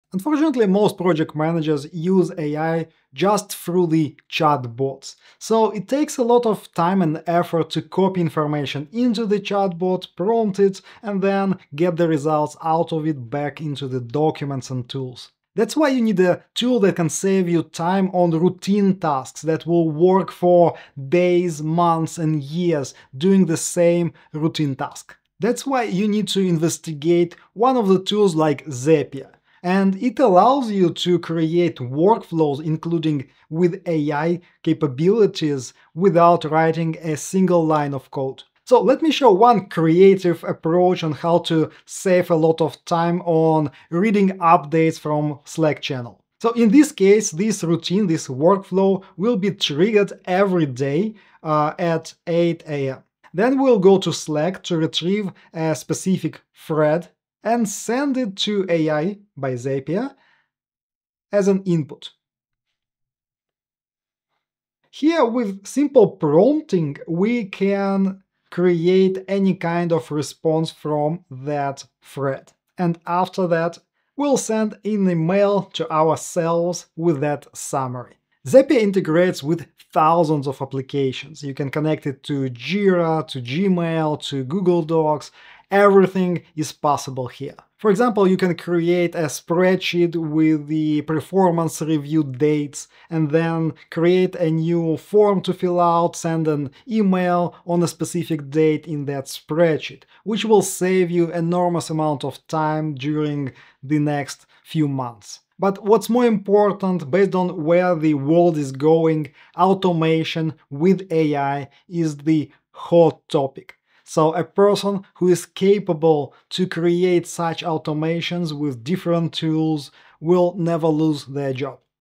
How Zapier Can Save You HOURS Every Week
Discover how most project managers are using artificial intelligence inefficiently! Learn about the power of automation and how it can streamline your processes. Explore how Zapier can save you time and boost productivity through workflow automation and AI automation.